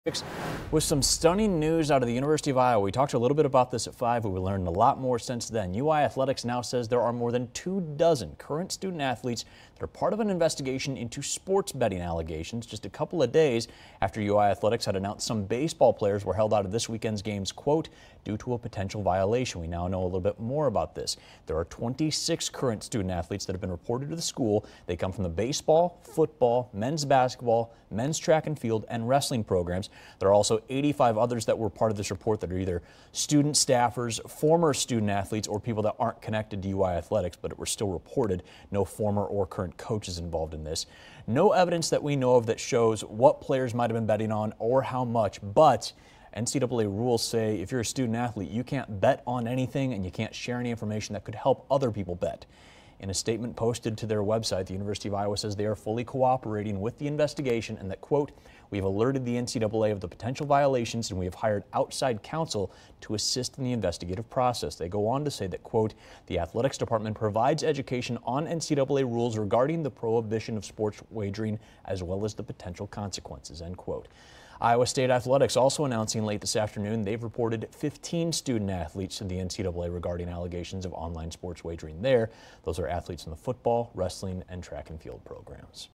Sports betting investigation underway involving 26 Iowa Hawkeye athletes
University of Iowa confirms there's a sports wagering investigation into 26 current UI athletes Monday. It includes members of the baseball, football, men’s basketball, men’s track and field, and men's wrestling teams, as well as one full-time employee of the UI Department of Athletics.